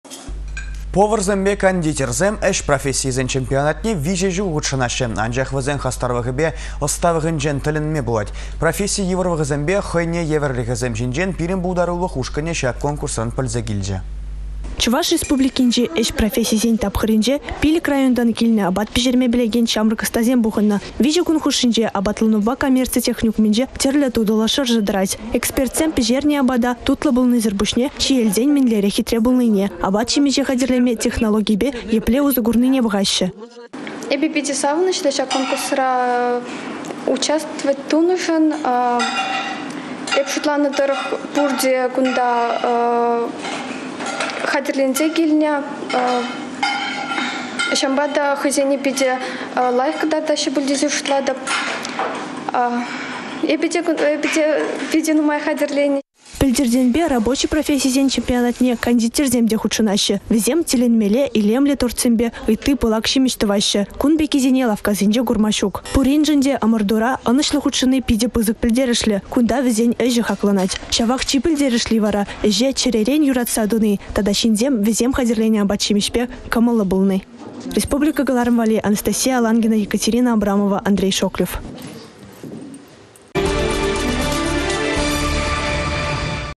Поварсемпе кондитерсем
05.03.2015 Поварсемпе кондитерсем ĕç профессийĕсен чемпионатне 3 çул хутшăнаççĕ, анчах вĕсен хастарлăхĕпе ăсталăхĕнчен тĕлĕнме пулать. Професси йывăрлăхĕсемпе хăйне евĕрлĕхĕсем çинчен пирĕн пултарулăх ушкăнĕ çак конкурсран пĕлсе килчĕ.